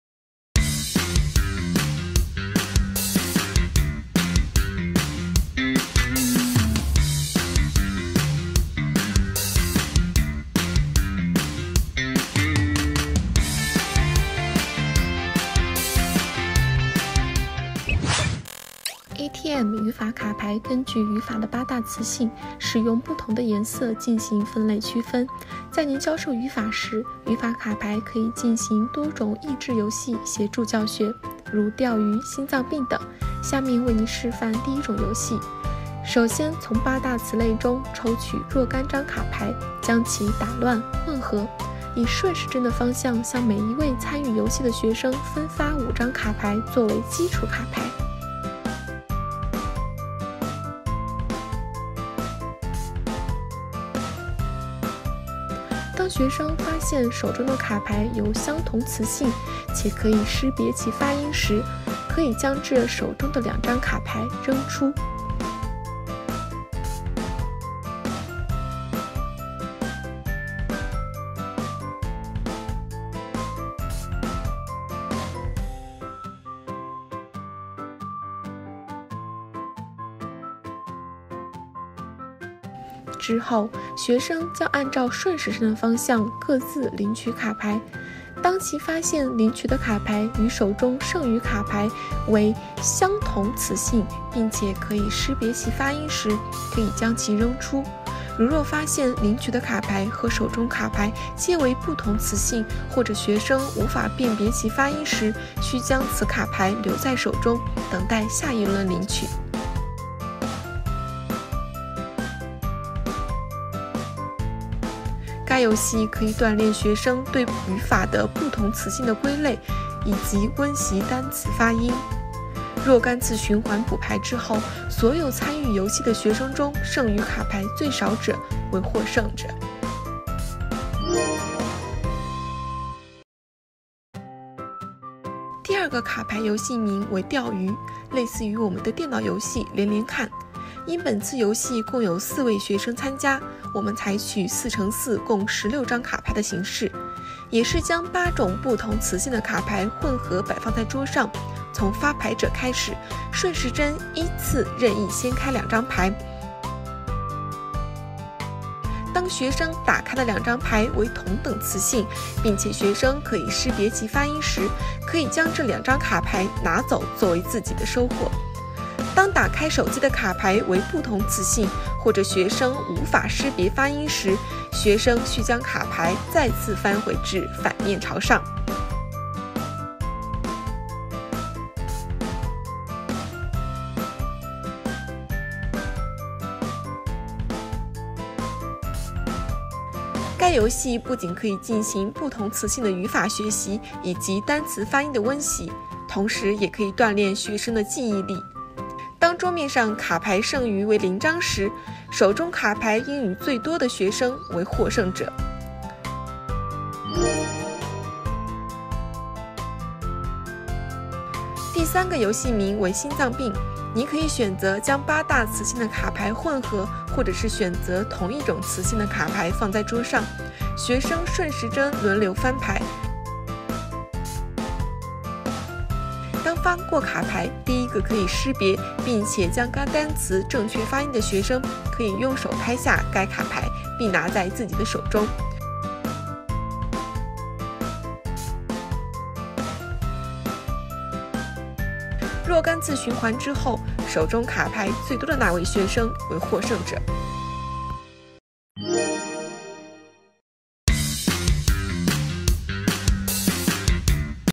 教具教學视频 | 法卡牌Grammar cards
語法互動小遊戲檢驗孩子語法成果
ATM將字母拼讀Phonics融合UNO Cards的概念，藉由遊戲設計讓孩子不僅僅是在玩遊戲，也在不知不自覺中練習字母拼讀的發音規則，卡牌有三種設計，分別為：Short Vowels, Long Vowels and Blends.輔助教學小卡片，在遊戲過程中建立學生對字母和發音的直覺音感。